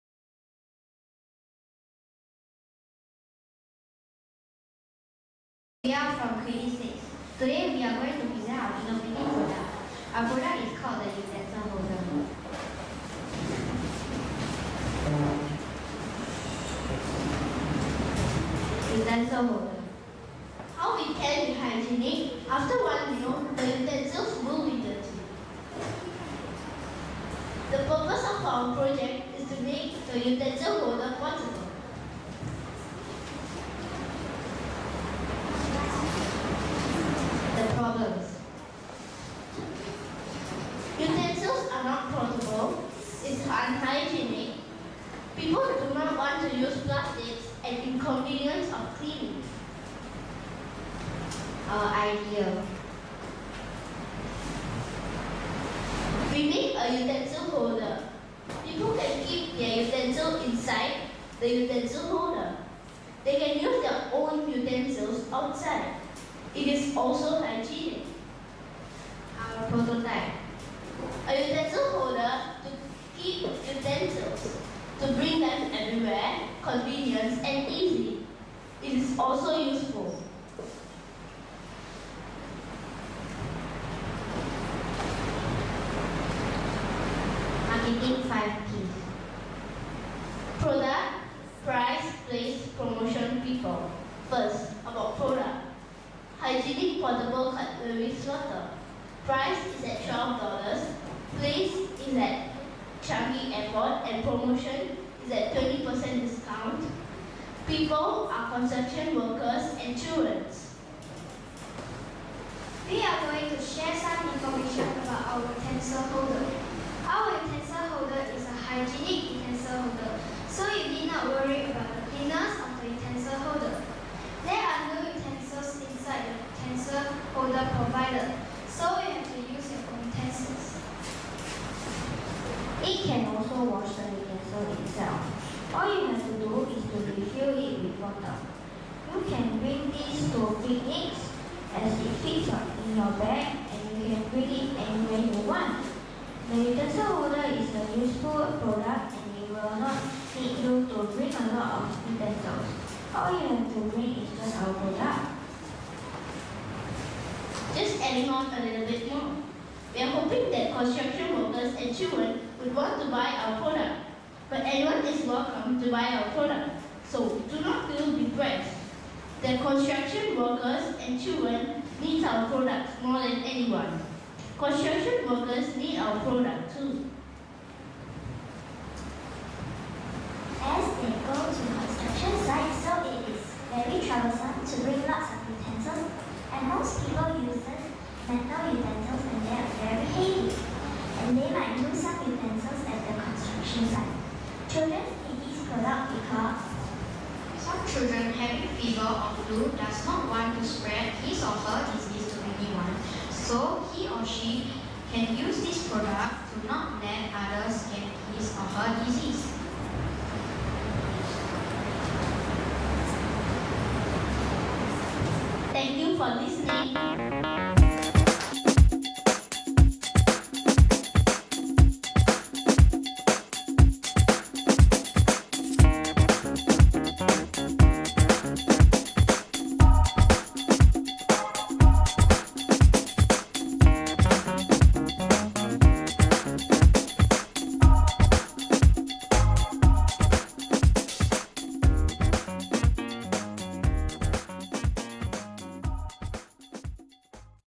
Woodgrove Pri Sch - Hygienic Portable Cutlery Slotter
Hygienic and Portable Slotter carrier that holds spoons, forks, knives and chopsticks (with self-cleaning mechanism). The purpose of our project is to make the utensil holder portable, clean and hygienic. The problem is that even though utensils are portable, it is difficult to find washing area to keep it hygienic. Do not want to use plastics due to its flimsiness and the inconvenience of cleaning.